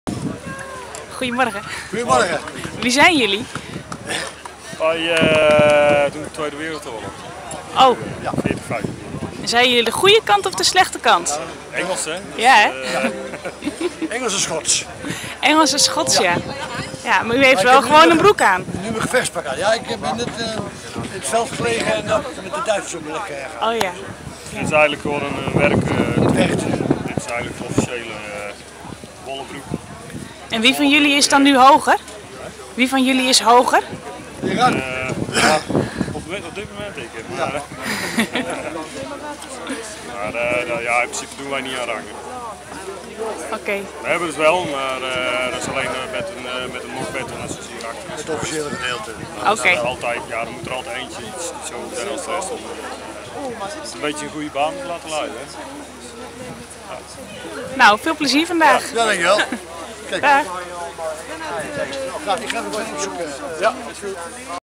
Wie heeft de hoogste rang? - Festival Levende Geschiedenis 2010
GeschiedenisBeleven.nl vroeg de deelnemers van het Festival Levende Geschiedenis 2010 wat ze aan het doen zijn. In deze video: Wie heeft de hoogste rang?